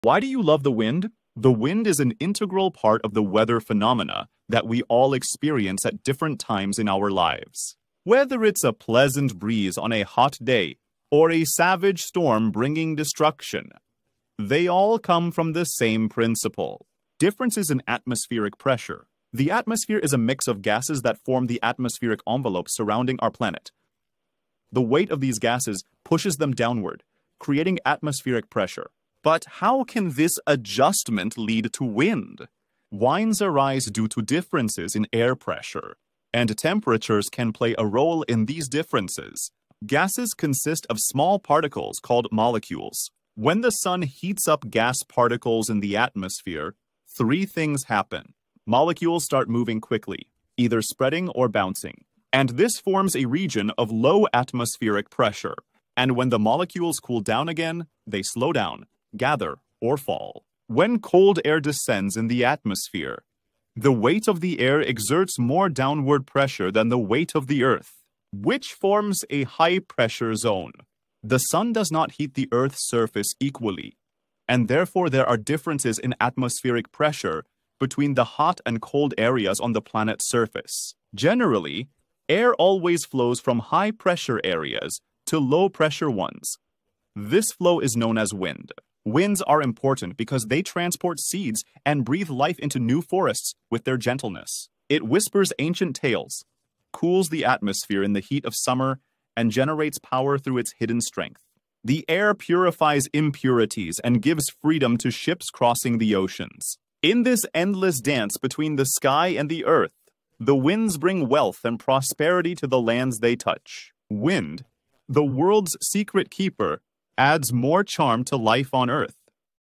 The Hidden Science of Wind: Why Earth's Breath Matters
The hidden science of wind reveals Earth's powerful atmospheric forces and their impact on our planet. Discover how differences in air pressure create the winds that shape our world and influence life as we know it.

Explore the fascinating journey of wind formation, from molecular movements to global weather patterns. You'll learn:
• How solar heating creates pressure differences in our atmosphere
• Why cold and hot air movements generate wind patterns
• The crucial role winds play in seed dispersal and forest growth
• How wind power is transforming sustainable energy production

From gentle breezes to powerful storms, wind is Earth's breath - a force that's essential for life, weather patterns, and our planet's delicate balance. This natural phenomenon influences everything from aviation to space exploration, while playing a crucial role in our sustainable future.

Have you ever wondered why some regions experience stronger winds than others? Share your wind-related experiences in the comments below!

CHAPTERS:
00:00 - Why do you love the wind
00:36 - How does wind form
01:20 - Importance of wind in nature